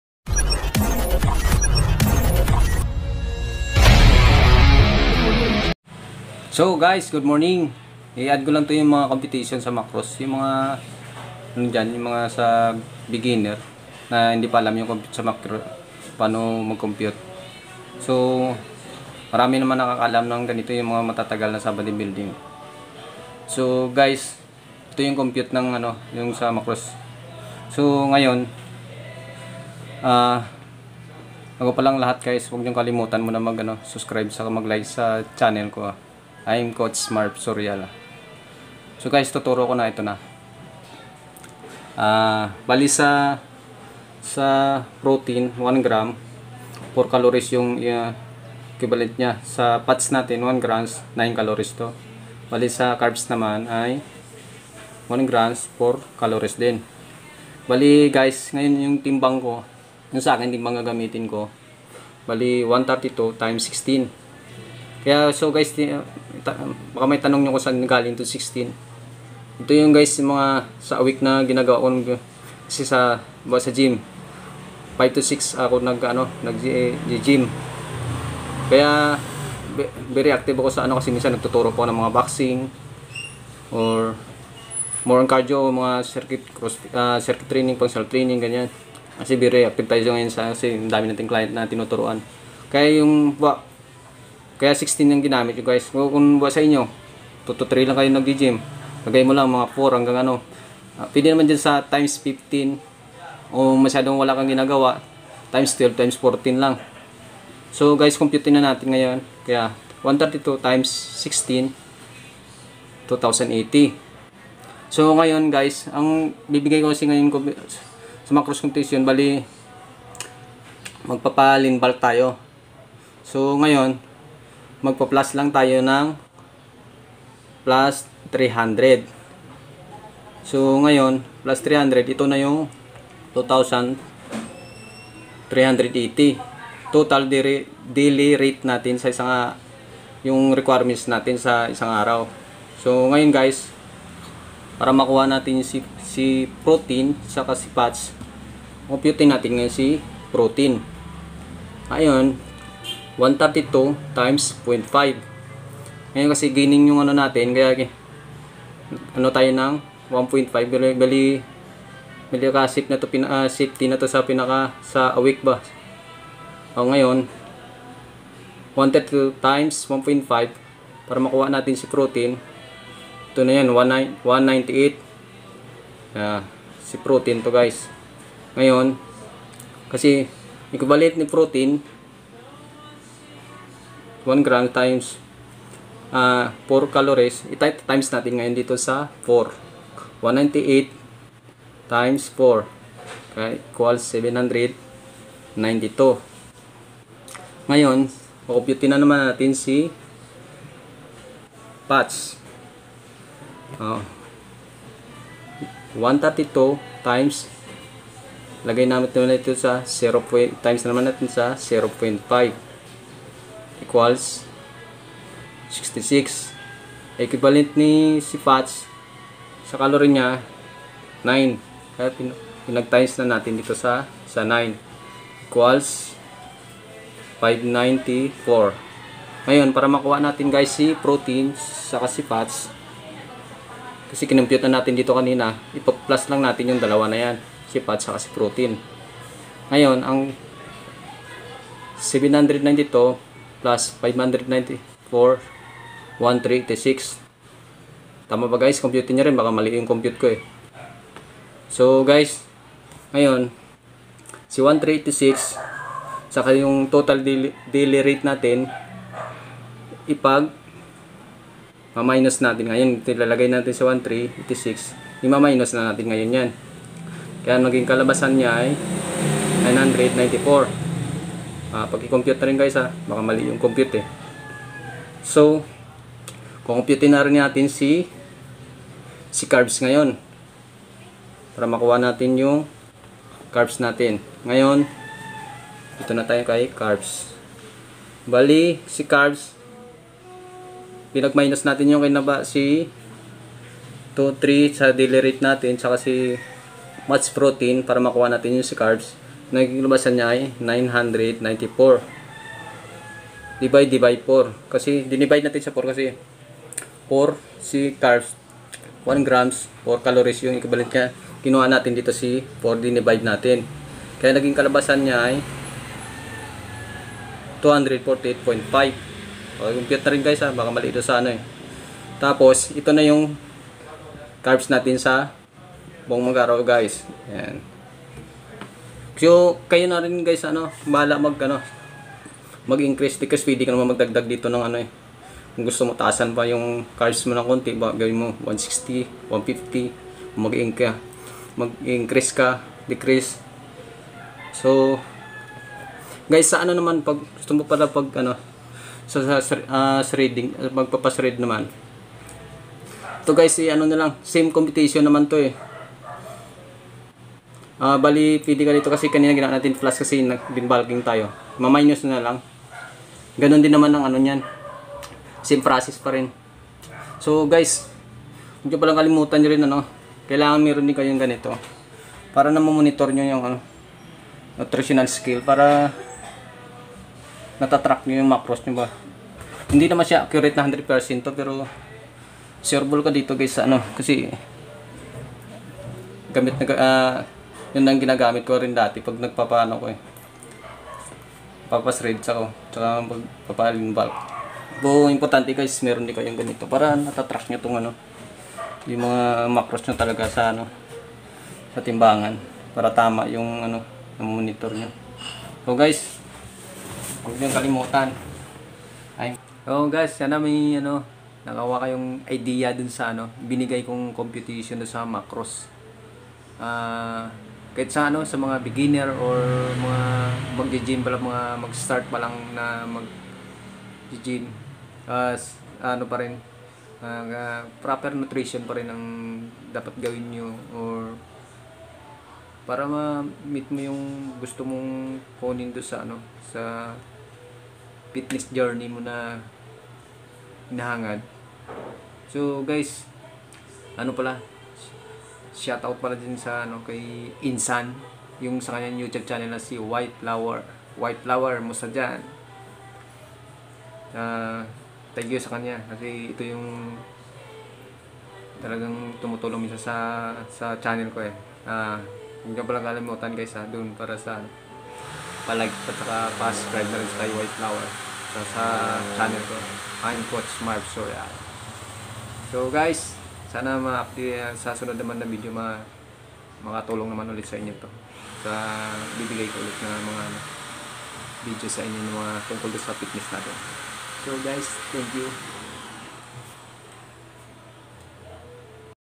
How to compute food macros (vlog#26)
fitness # Sports # travel

Lean bulking diet,
Lean bulking tips,
Lean bulking macros

If you're counting macros for weight loss, you'll want to make sure you're counting macros in such a way that you're also cutting calories. Try this range of macro ratio for weight loss: 10-30% carbs, 40-50% protein, 30-40% fat. Then adjust accordingly.